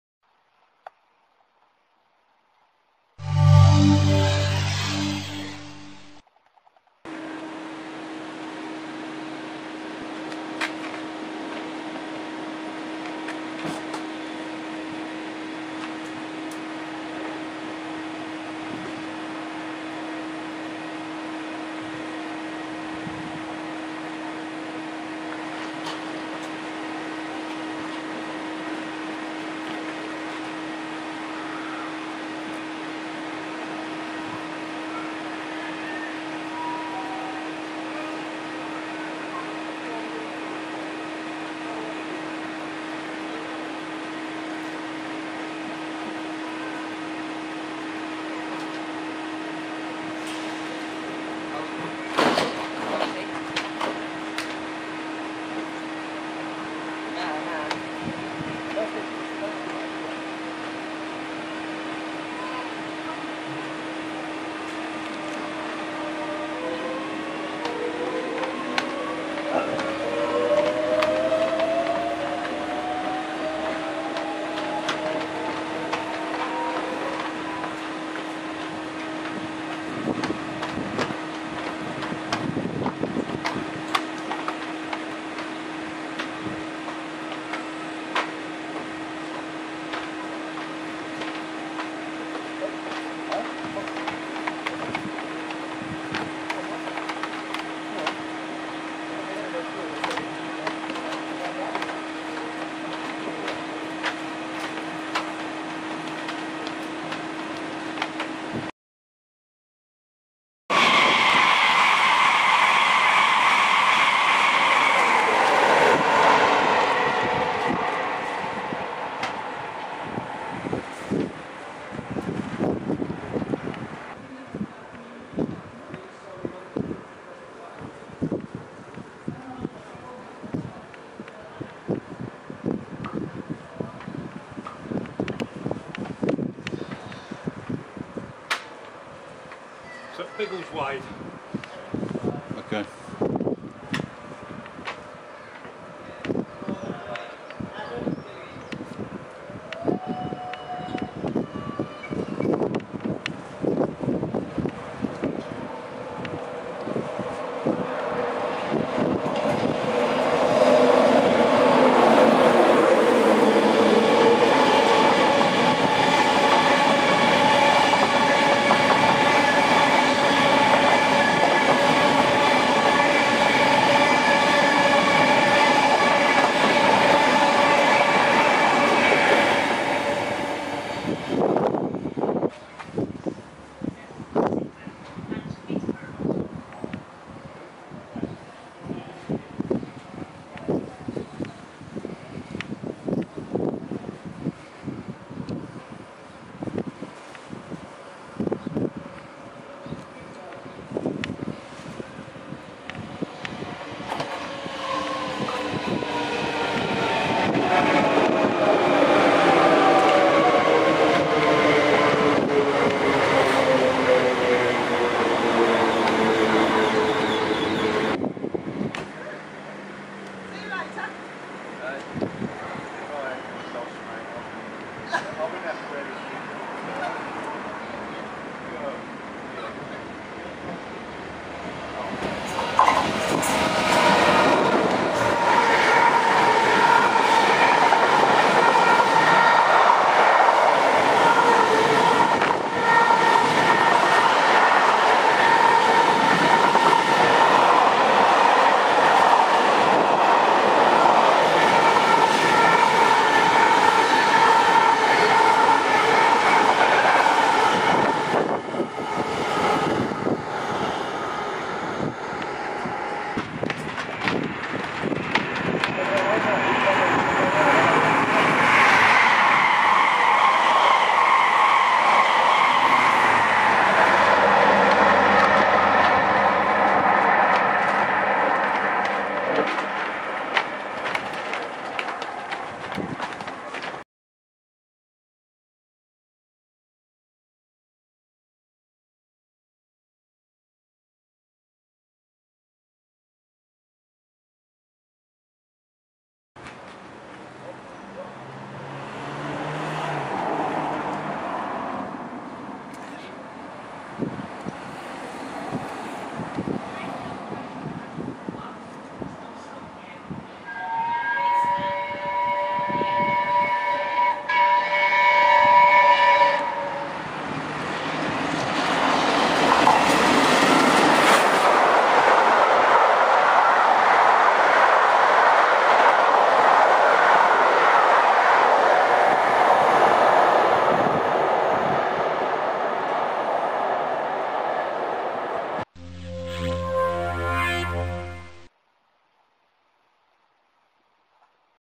18-05-2012 No 60163 Tornado.mpg
Video diary of No 60163 Tornado working the second leg of The Cathedrals Explorer between London Victoria and Durham on the 18-05-12 at St Neots.